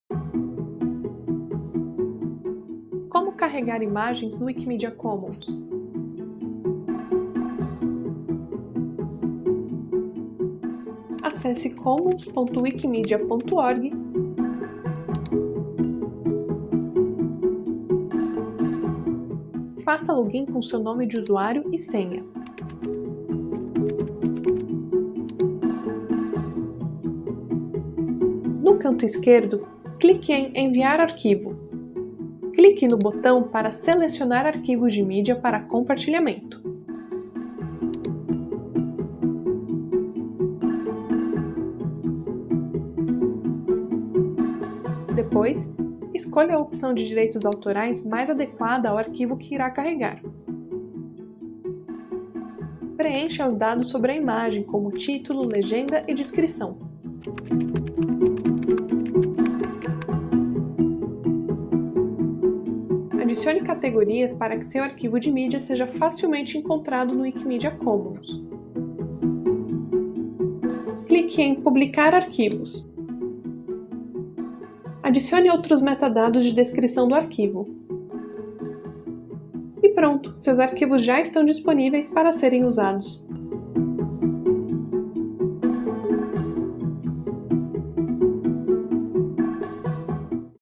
Como carregar imagens no Wikimedia Commons
Vídeo tutorial sobre como carregar imagens no Wikimedia Commons.

Autoria: Wiki Movimento Brasil
Roteiro: Éder Porto
Ilustração/animação: Daniela Boyadjian
Narração: Érica Azzellini